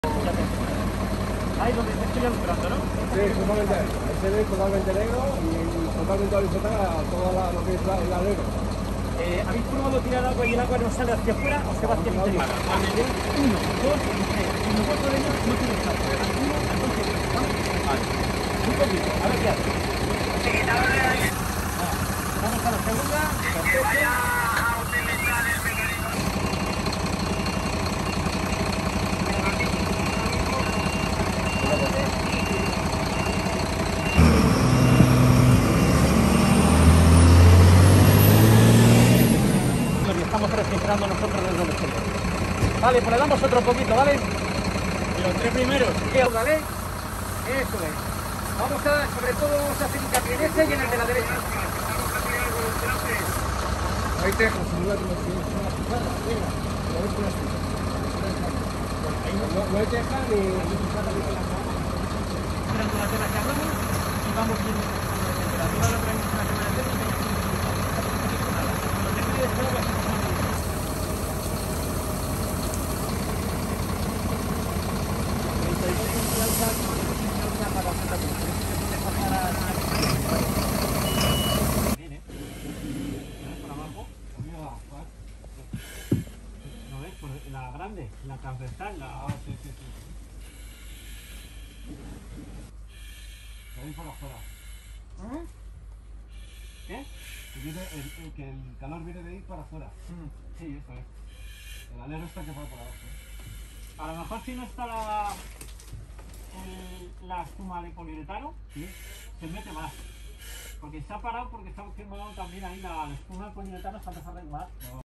Incendio chimenea Villamantilla
Pequeño incendio de cubierta rápidamente controlado por BomberosCM. El dueño de la vivienda resulta intoxicado leve y es atendido por SUMMA112. interviene también Guardia Civil